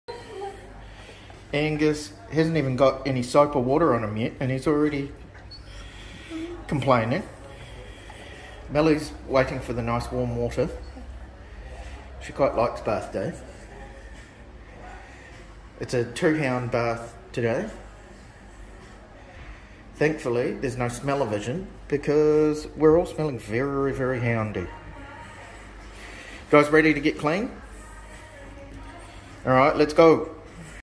Basset hound bath day
Millie and Angus are not so patiently waiting for their bath.